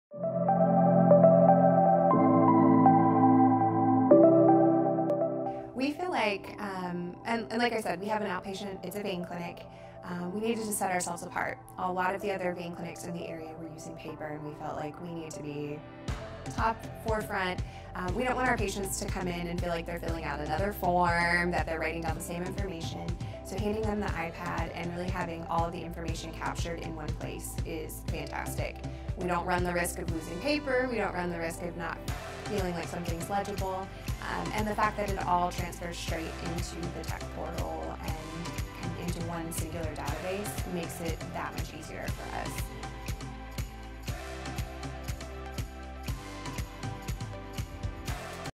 Creating a Paperless Front Office - Nicole Brown - MBB Radiology
Royal's Patient Kiosk solution allows you to create a "paperless" pre-registration and check-in workflow